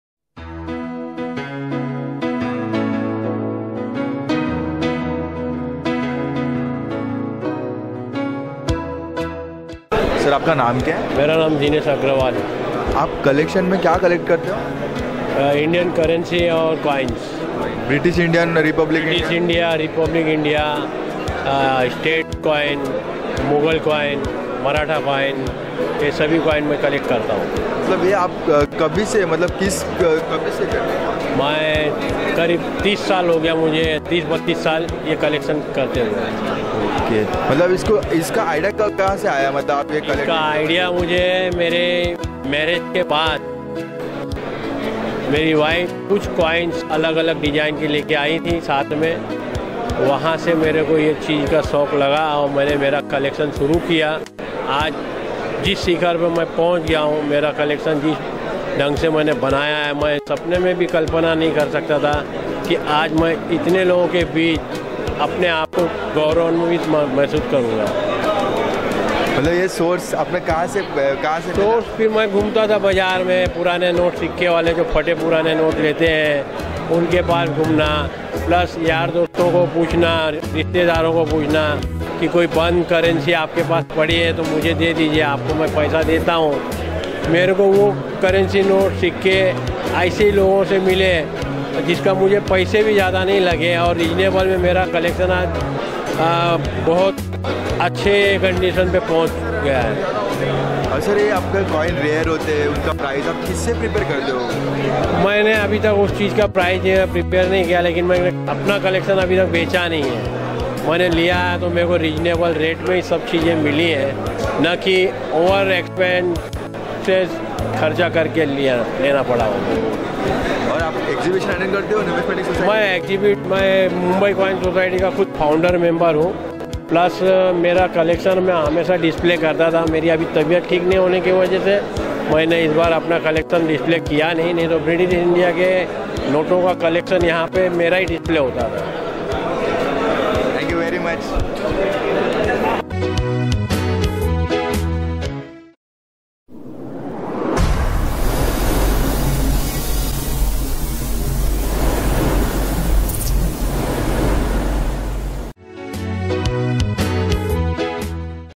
Mr Dinesh Agrawal Interview @ MCS Numismatic Fair 2014 - ICCG News
by - "Indian Coins & Currency Group " India's Largest Numismatic Community . ICCG NEWS

#‎Save Coins Save Heritage #‎Be Numismatic